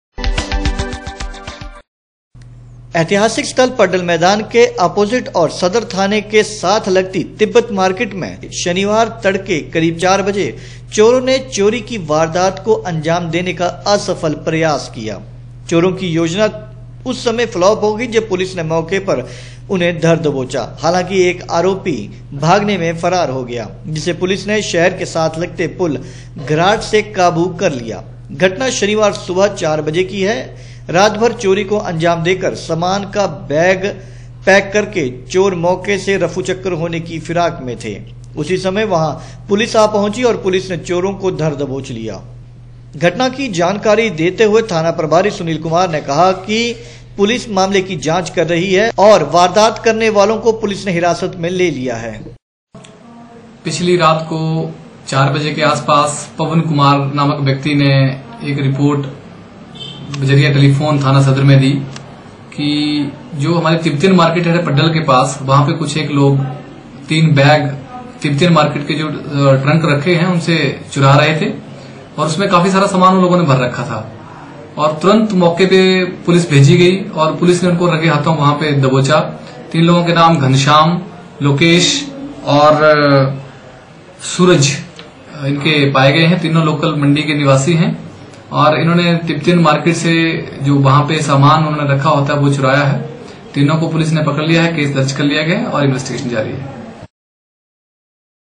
सदरथाने के साथलगतीतिब्बतीमार्केट से चोरों ने चोरी की वारदात को अंजामदेने का प्रयासकिया
सदरथाने के साथलगतीतिब्बतीमार्केट से चोरों ने चोरी की वारदात को अंजामदेने का प्रयासकिया है चोरों की यह योजनाउसमें धरी की धरीरहगईजबपुलिस ने मौकेपर धरदबोचा